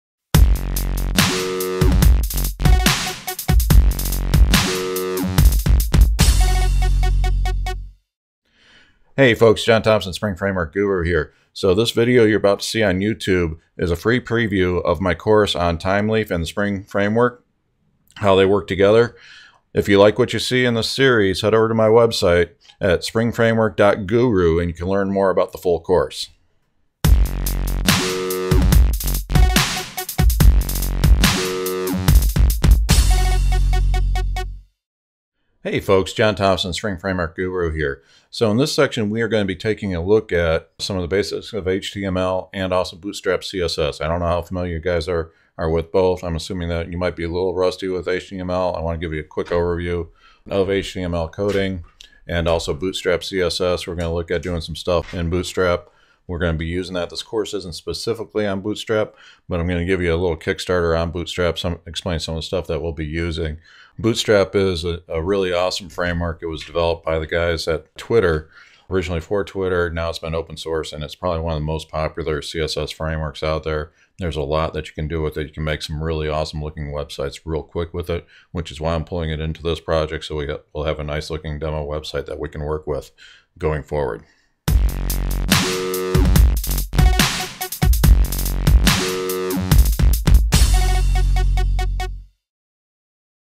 2.1 - Bootstrap CSS Section Overview
Learn how to use Spring Boot with Thymeleaf!

Overview of the Bootstrap CSS portion of my Mastering Thymeleaf Spring Boot Course.